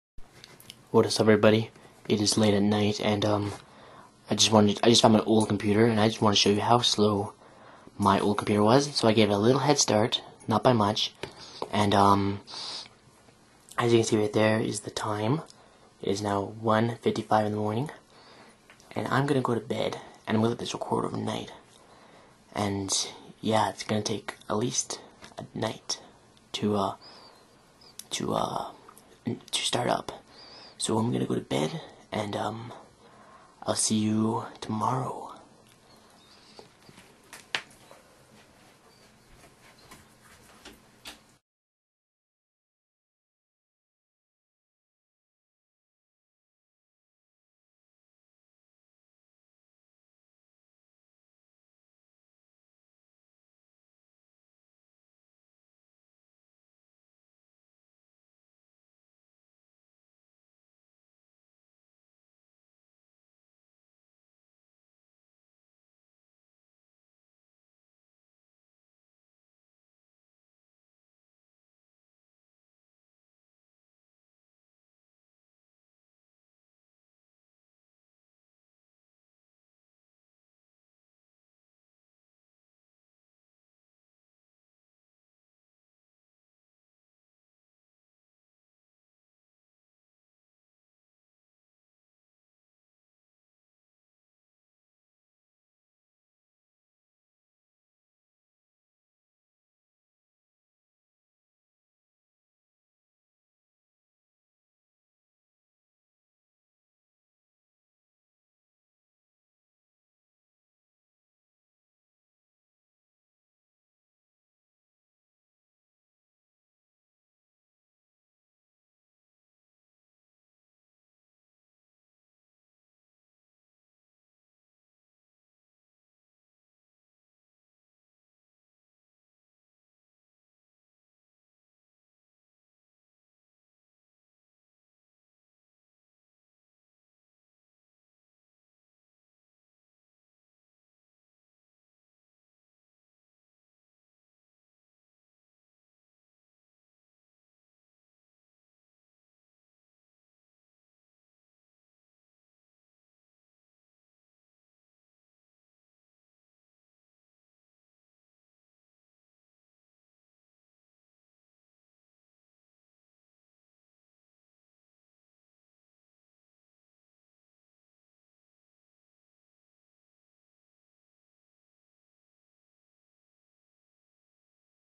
World's Slowest Computer! I SWEAR!
Sorry About The Suck Ass Quality, I Did That For Faster Uploading Speed...
So I Was Waken By The DING Of My Camera Yelling: NO MEMORY!
And If You Wanna Know, I Didnt W8 For It To Finnish Booting...
Btw... I Was Using My Internal Memory, If You Ask Why I Could Only Capture 20 Mins...